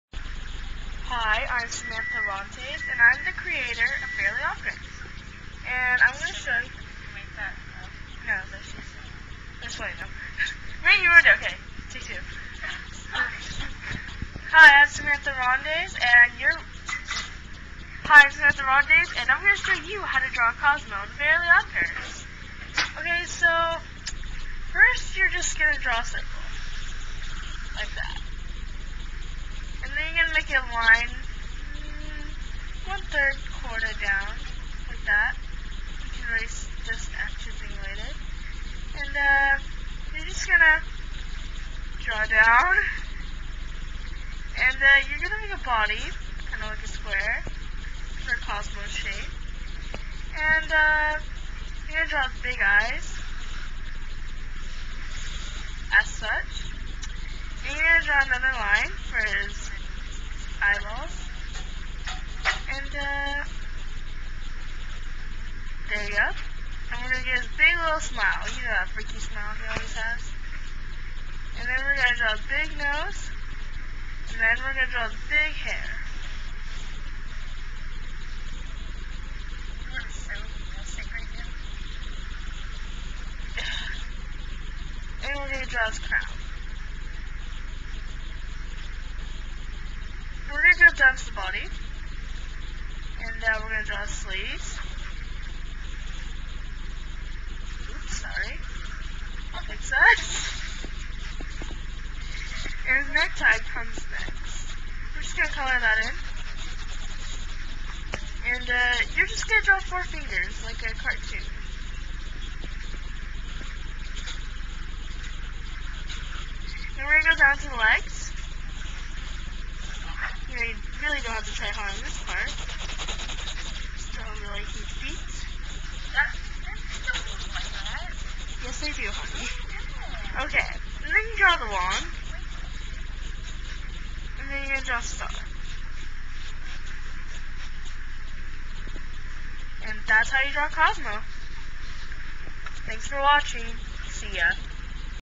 How To Draw Cosmo-Spoof!
Okay, of course this is fake haha. and it was hard to draw because I was holding the webcam in one hand, and drawing with the other which makes it even more hilarious. The person in the background is Sherri. and the name I say in here is not real XDD!